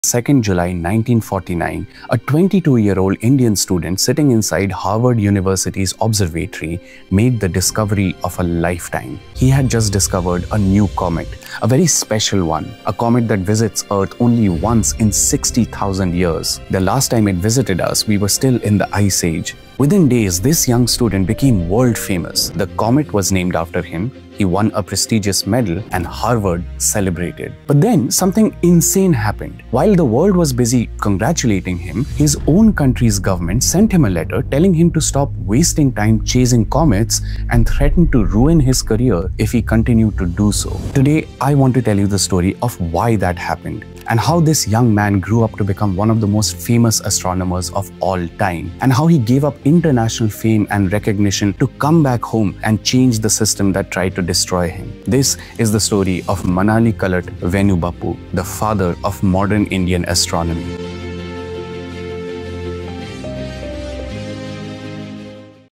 He discovered a comet but his own country punished him
Discover the fascinating story of an Indian scientist who made a groundbreaking discovery in astronomy, only to be punished by his own country. This unrecognized talent in the field of space exploration had achieved something remarkable, spotting a comet that would change our understanding of the universe. Despite being a superstar in the world of astronomy, his contributions were not initially recognized in his homeland, India.